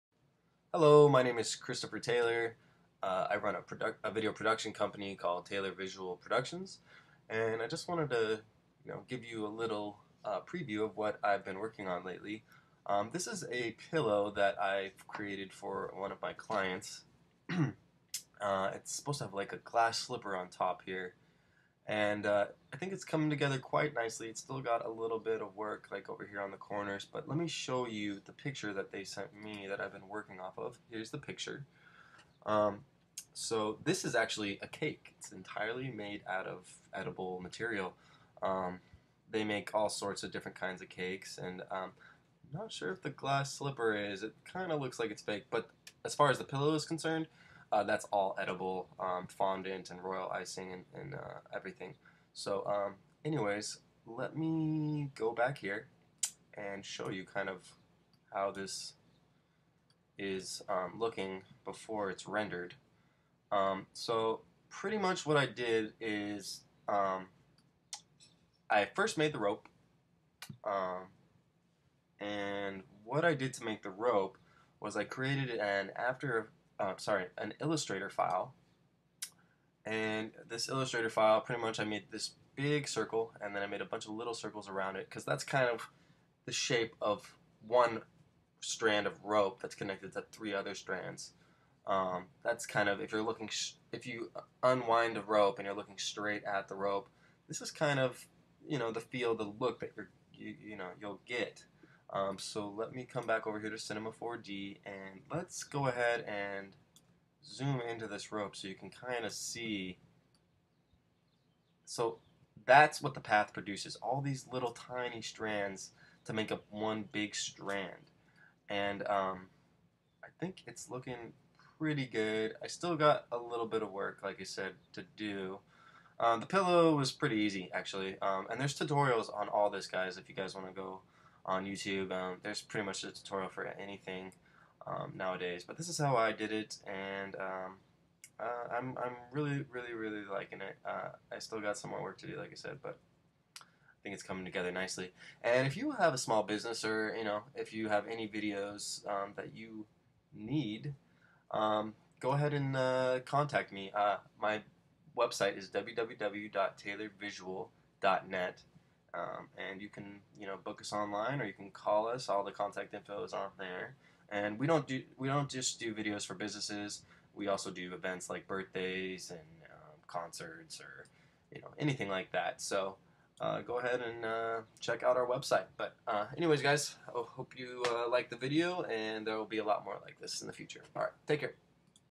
Cinema 4D I 3D Pillow & Rope Models
Taylor Visual's latest 3d animation work for our client YummyArts.